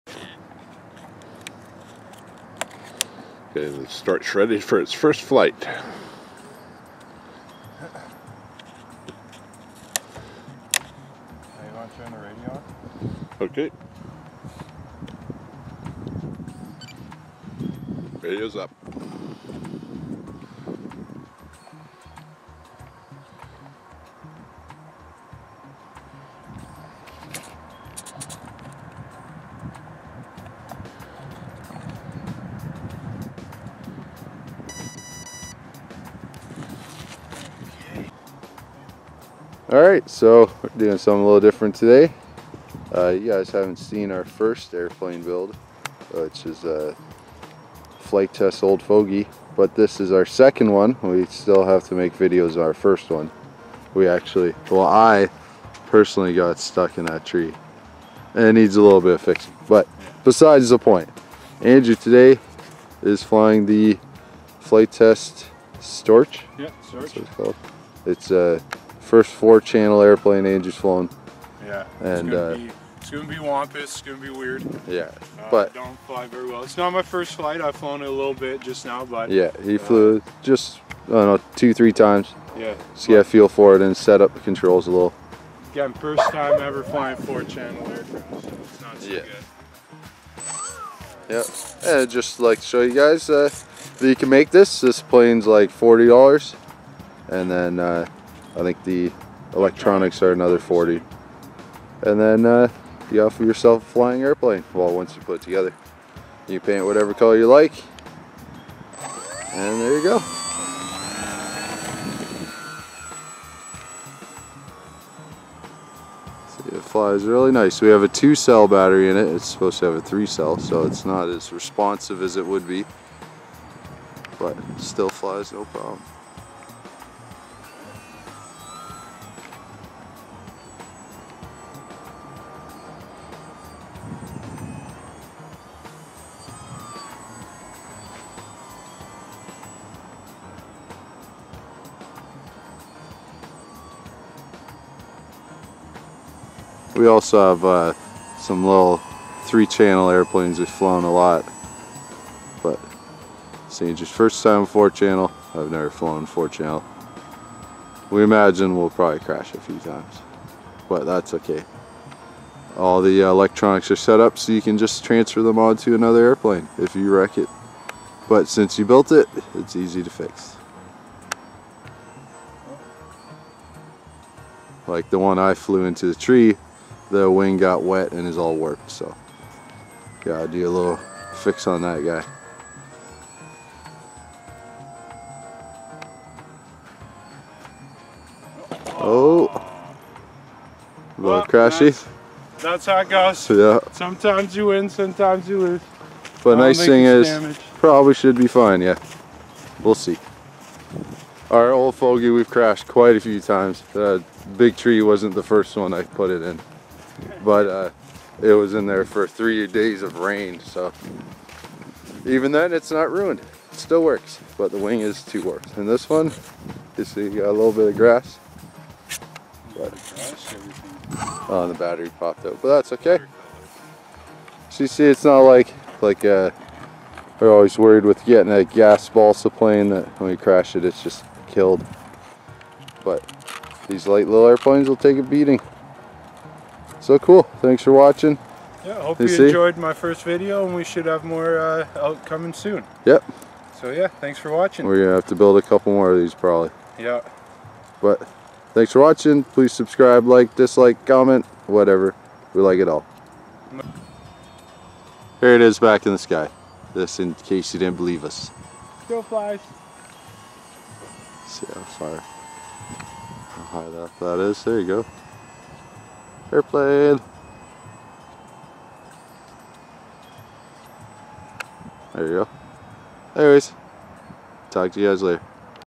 First flight ( and crash ) of our Flite Test Simple Storch
Just a short vid of our Simple Storch kit we purchased from Flitetest. They sell this kit for $39 US. The plane is made from 5mm foam board and you assemble the pieces using a hot glue gun and "of course" glue sticks. We have two of their kits and plan to buy more. The kits are easy to make and more important easy to fly. The planes are quite robust and can take abuse but the best part is if you break some piece you can just cut another part from some foam board and hot glue it back together. The board itself is inexpensive and can be found at the local Dollar store. We also added an Orange RC RX3S Flight Stabilizer from HobbyKing to help stabilize the plane as this is the first four channel airplane any of us have flown, We purchased the power pack (Prop, ESC, Motor and Servos) from Great Hobbies for around $40 US.